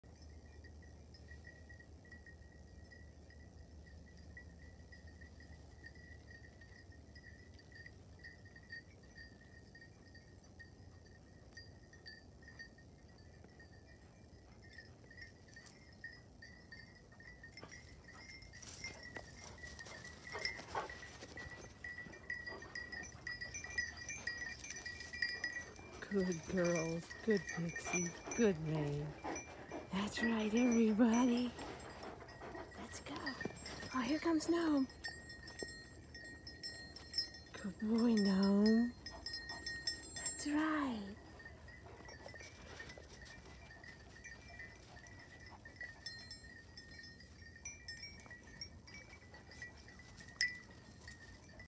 9 Belled Dogs on a Walk - Part 4
The beautiful sounds of the bells are more noticeable since the dogs are together after the initial excitement and running around.